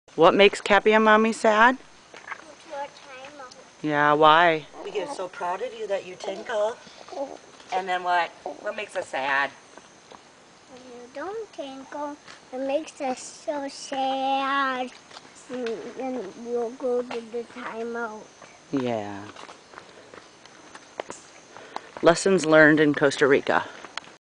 What makes us soo sad
Simon begins to understand the complex emotions and consequences behind potty training